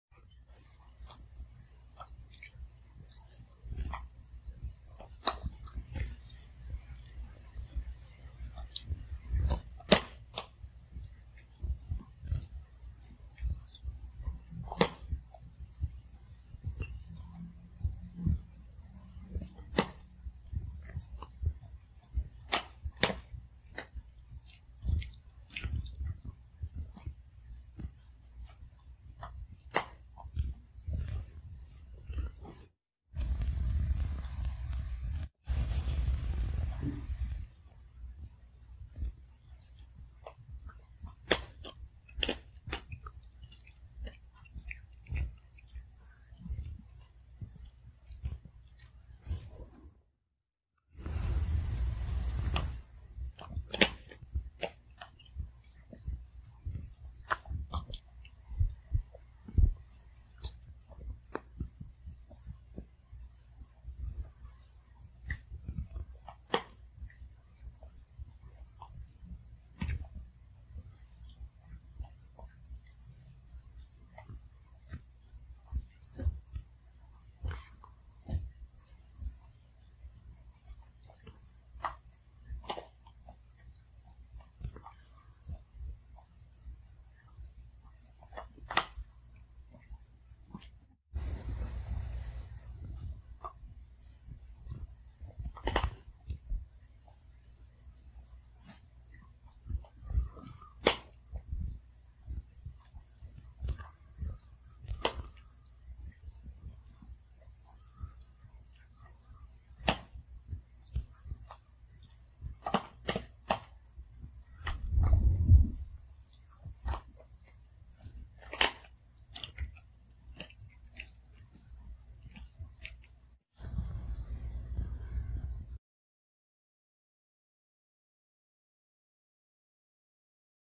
[죠니네] 특이하게 밥 먹는 강아지의 소소한 ASMR
한 알 한 알 꼭꼭 씹어먹는 예쁜 슈가
웬일로 고요~하고 평화~로운 우리 집..^^ 다들 합죽이가 됩시다 합!